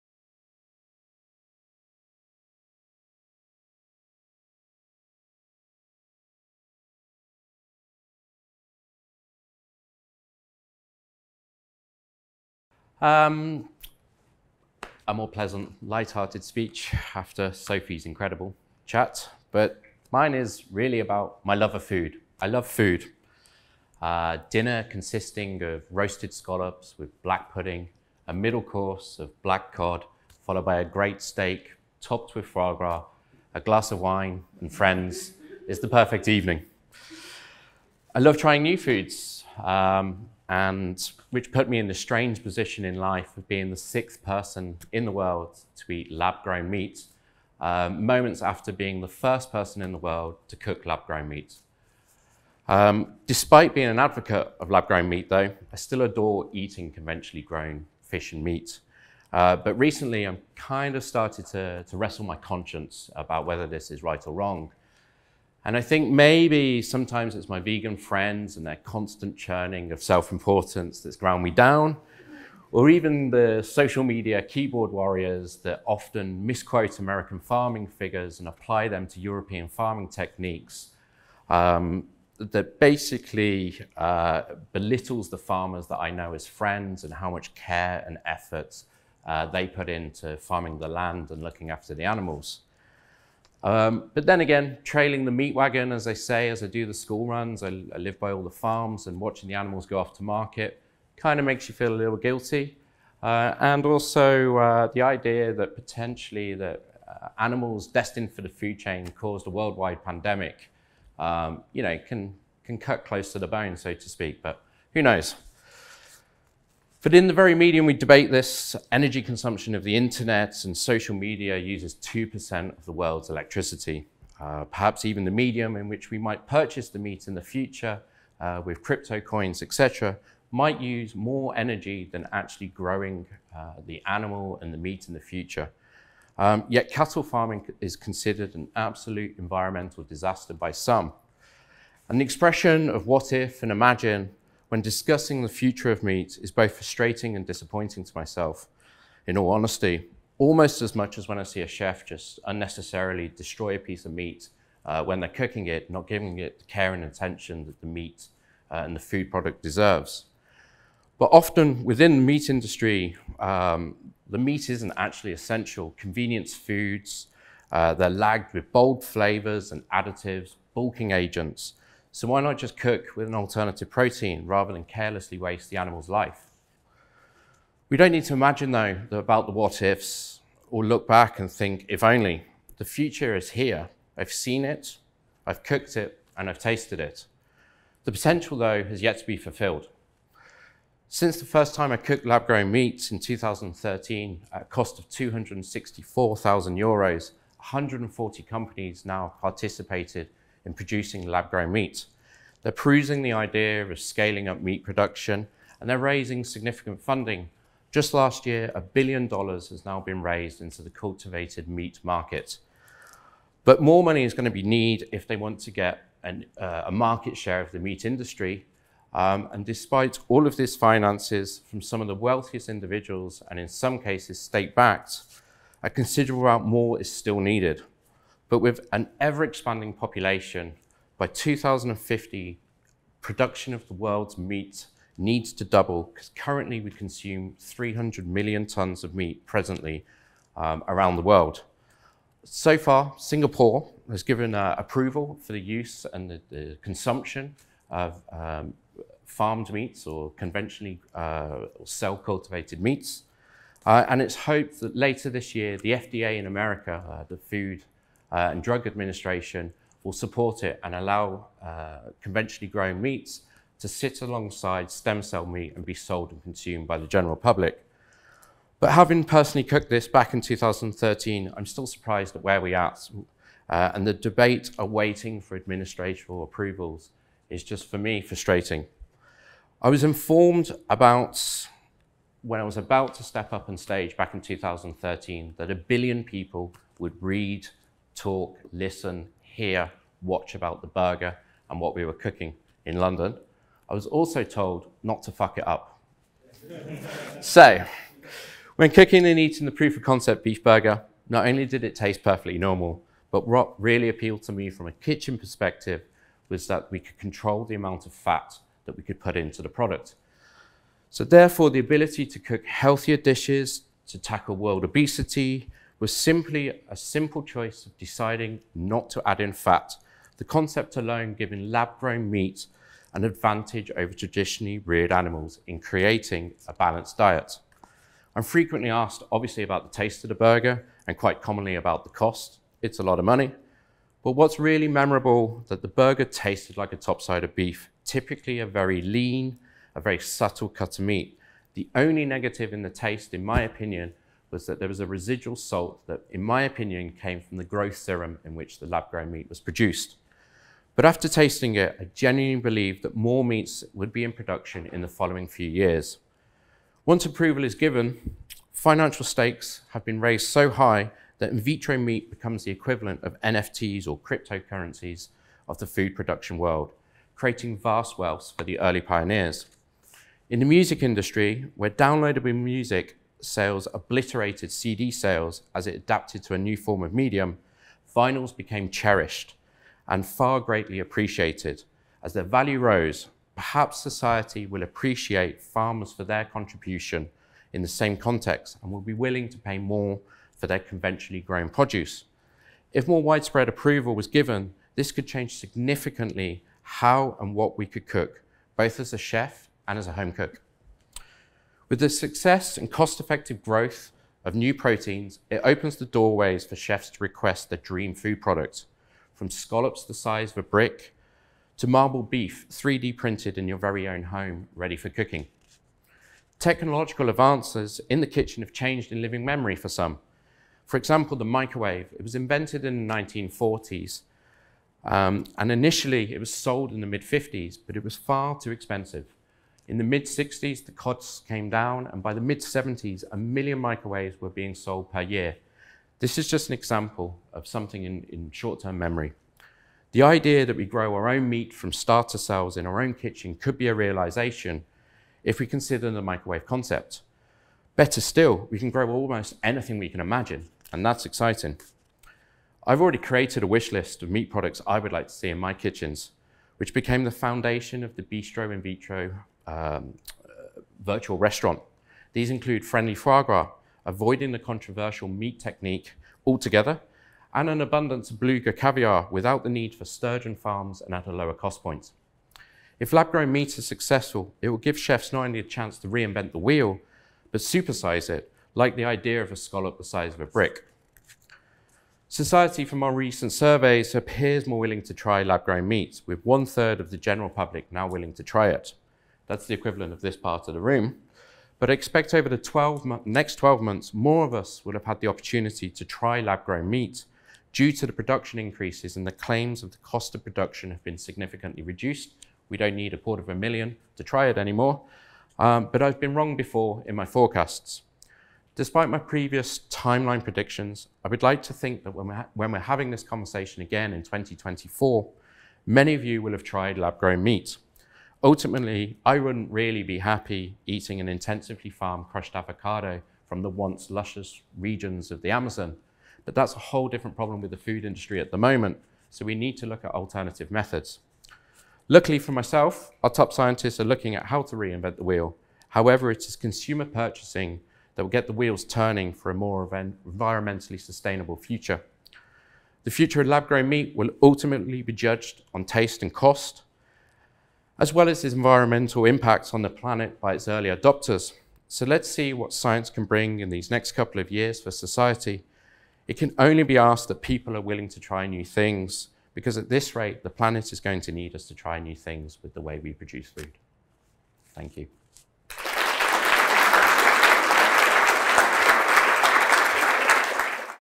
Reinventing meat production | Richard McGeown | TEDxHotelschoolTheHague
Richard McGeown shares his experience of preparing and tasting in-vitro meat. He gives an insight into the history and his prediction for the future of lab-grown meat and explains its environmental benefits. Also, he focuses on the health benefits resulting from the opportunity to grow meat with less fat and more nutrients. He leaves the audience hungry for more food for thought and what science will bring in the upcoming years. Richard McGeown is a British chef and restaurateur who cooked the very first in vitro meat product in history back in 2013.

His talk will be focused on the scientific & economic progress of lab-grown meat since 2013. He will discuss how the human impact on the planet needs us to consider how our food may be alternatively produced in the future, and whether this is socially accepted and economically viable, as demand for meat grows with an expanding population. This talk was given at a TEDx event using the TED conference format but independently organized by a local community.